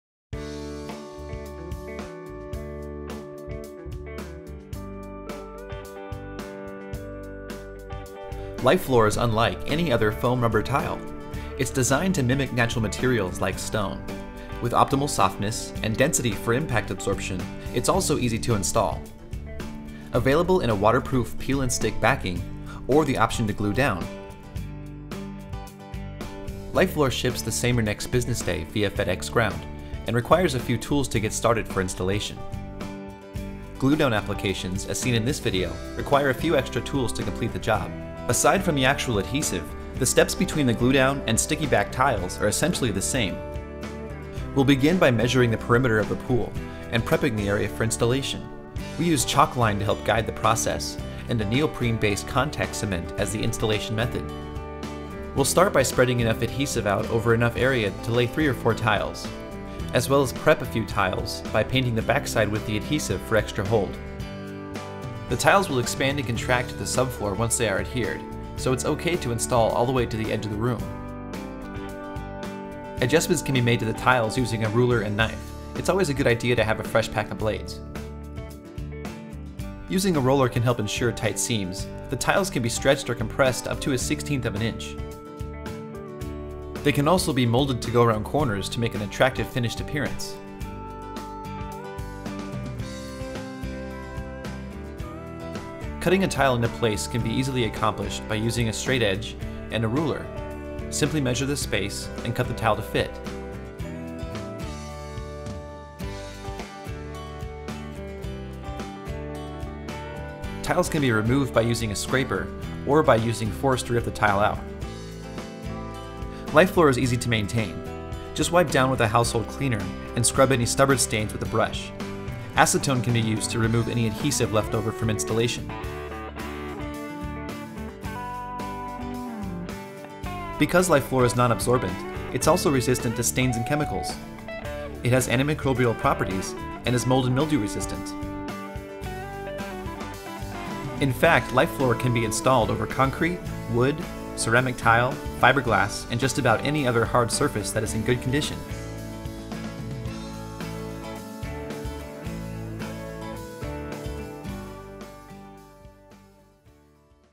How to Install Life Floor Slate Tiles
Life Floor Slate Tiles are the perfect solution for providing flooring around pools, on decks, and patios where water may be present. These tiles, unlike other pool and deck flooring solutions, are crafted from both foam and rubber which not only gives you a soft floor but one that also is very slip resistant.

On top of these outdoor tiles being soft and slip resistant, they are also cool to the touch in direct sunlight and will keep their color in direct sunlight for the life of the product. The tiles are also anti-fungal and anti-bacterial making them perfect for locker rooms, bathrooms, showers, and many other areas where a safe flooring surface is needed.

These tiles can be easily installed with the use of contact cement, double-sided tape or a polyurethane adhesive and can be installed underwater. With 7 different colors to choose from and a 5-year warranty, your pool, deck or patio floor are sure to be the talk of the neighborhood.